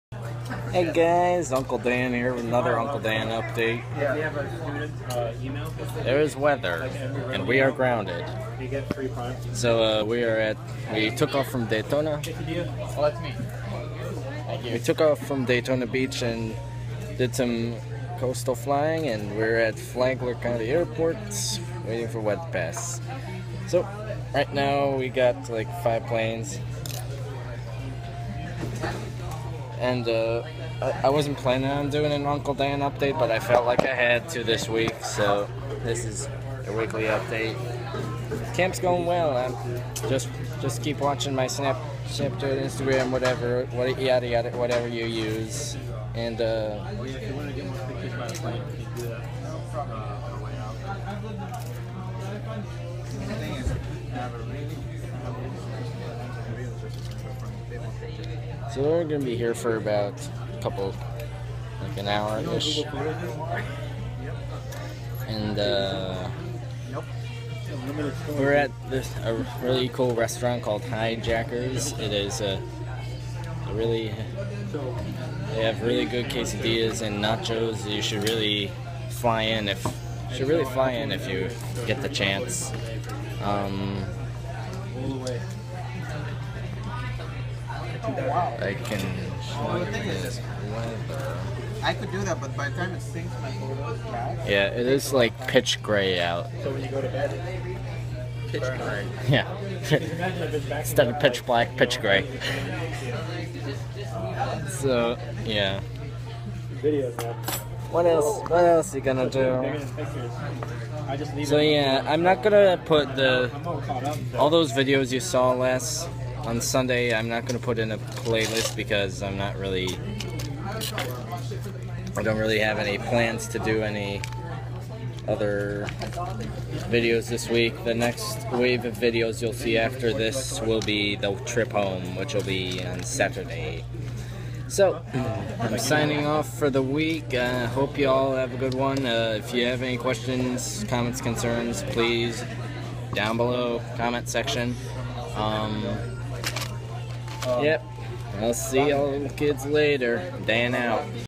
Uncle Dan Update- grounded. Literally.
Weather headed east. The storm looks angry, albeit small. 5 C172s landed at Flagler Co. Airport. A good majority of the parking spots were filled already.